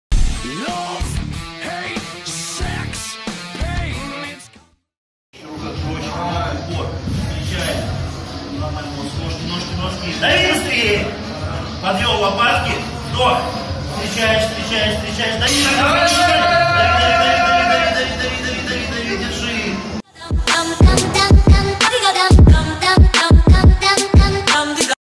Ключников Евгений - Жим 190х2
Ключников Евгений жмет без использования экипировки 190 кг на 2 повторения. Собственный вес спортсмена 91 кг.

Чуть больше двух месяцев назад Женя пожал 180 кг без экипировки на Чемпионате России WPA в Нижнем Новгороде, это был его личный рекорд